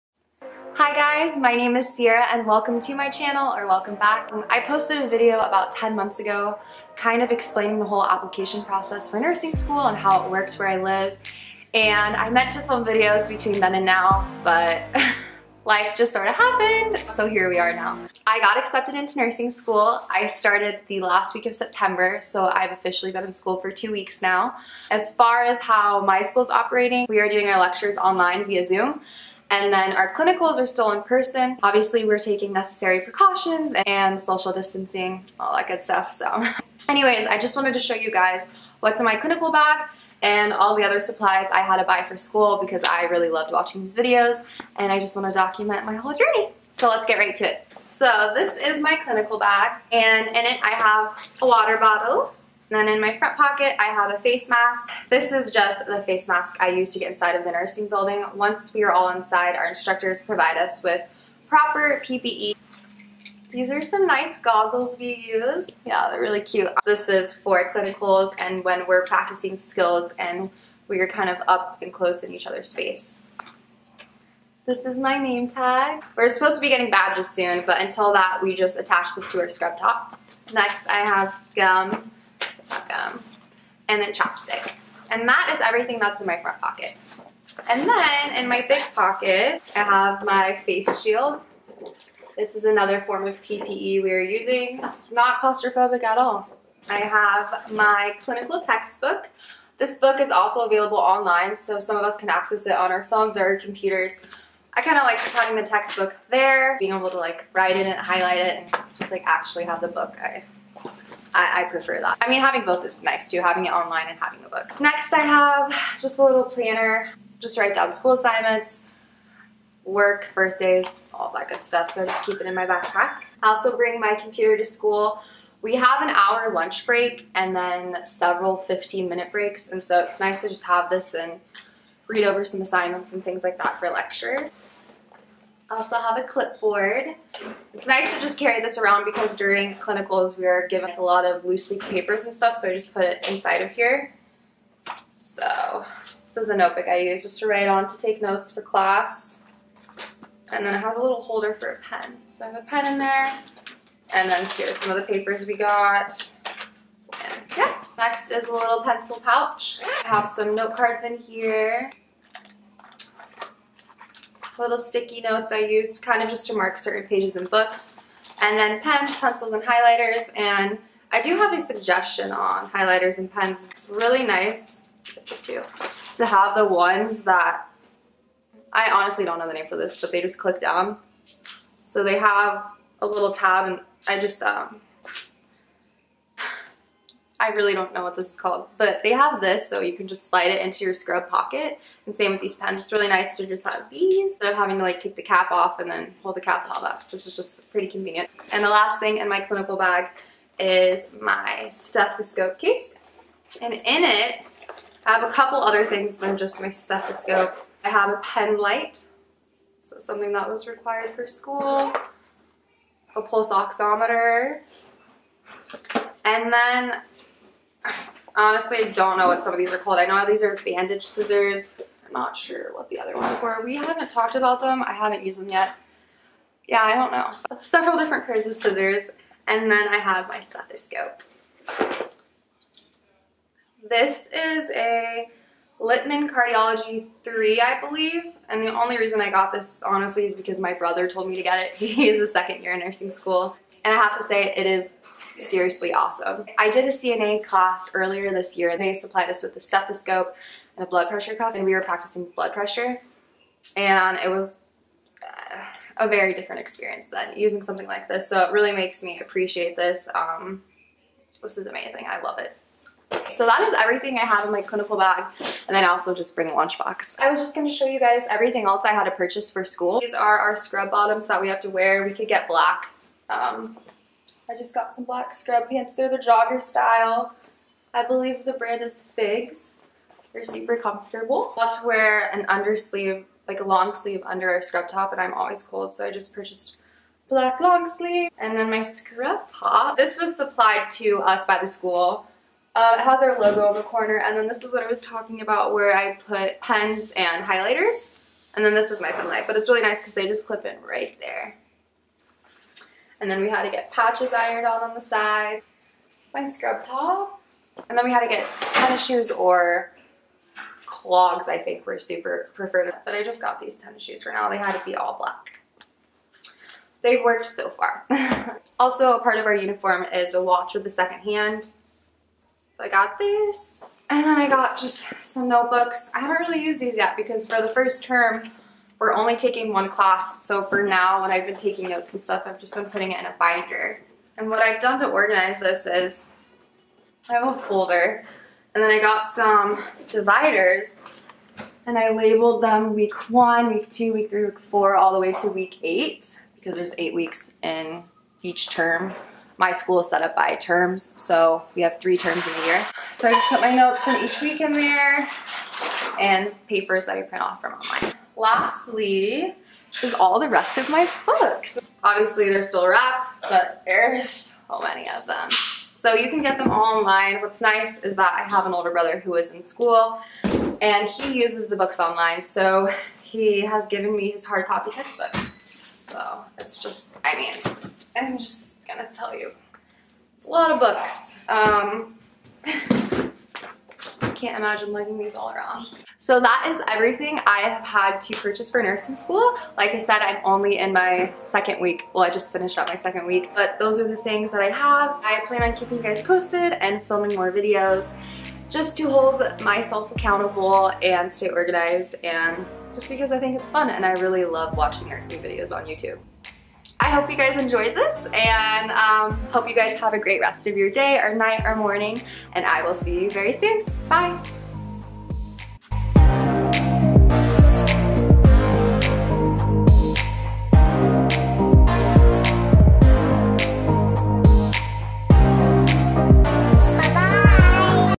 NURSING SCHOOL SUPPLY HAUL
Hi friends! I am so so excited to begin this amazing journey and to share my experience with you all! Happy October!